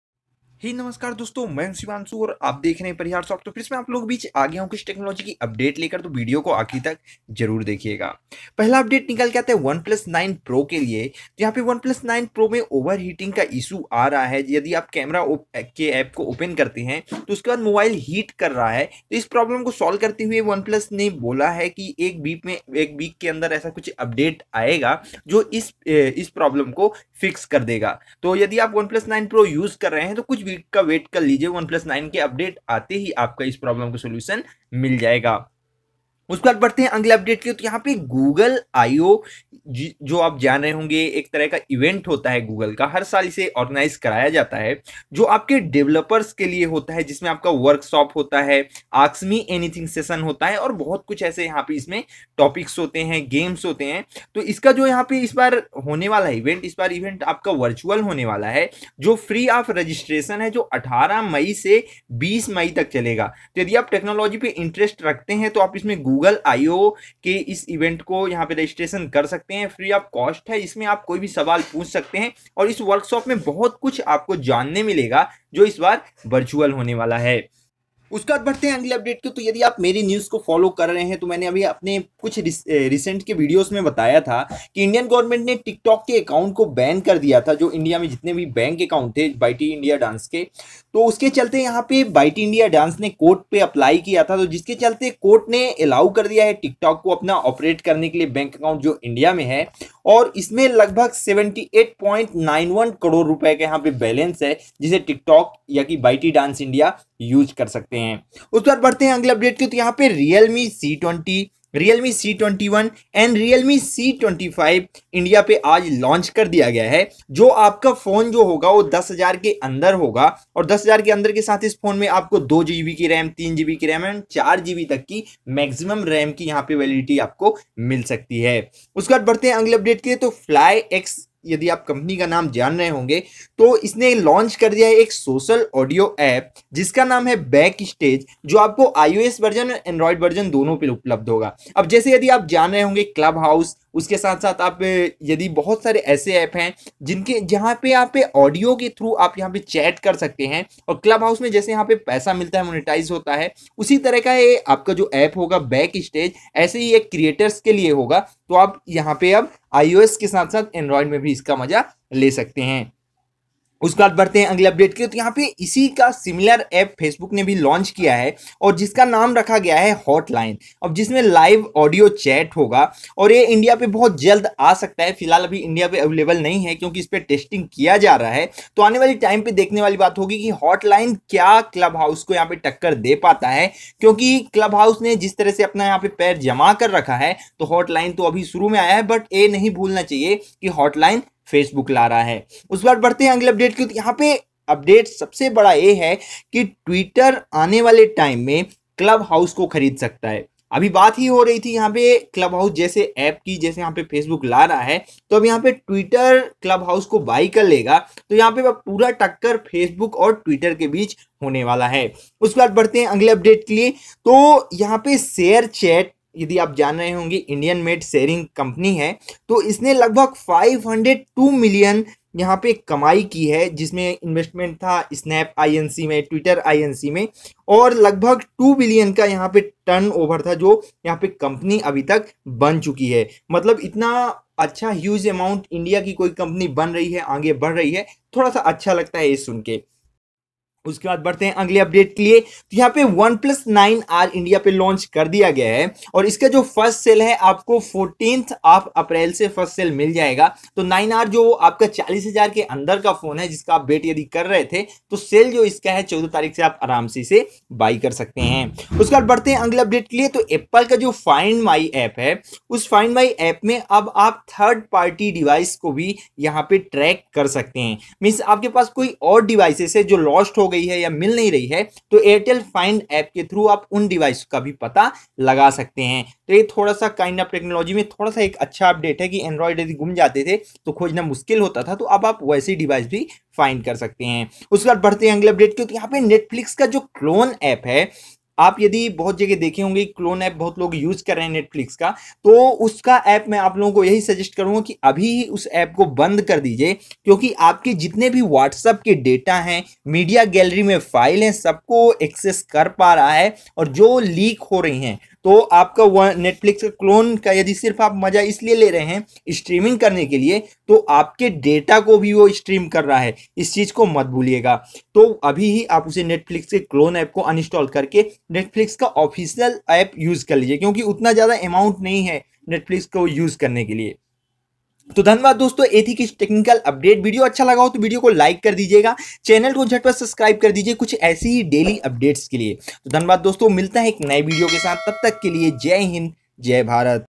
Twitter buy clubhouse,Netflix scam,Facebook hotline,1+ 9pro overheat,Google Io event : Tech Updates
Tech Update in this video:
Oneplus 9pro overharting issue why?
Google Io event details
Court on Tiktoc ban bank account.
Realme C20 , C21 , C25 Launched
FLYX launches Social audio app named Backstage
Facebook Lauches simliar app like clubhouse hotline
twitter buy Clubhouse
Sharechat Raises $502 Million
Oneplus 9r launch and sale date
apple Find my app for 3rd party devices
Netflix clone app is danger

1Like=Love
1Subscribe=Family
1Share = Support

Love, Family & support

LINKS► Nahi h bhai kyuki paid video nahi h

Cheers
pariharsoft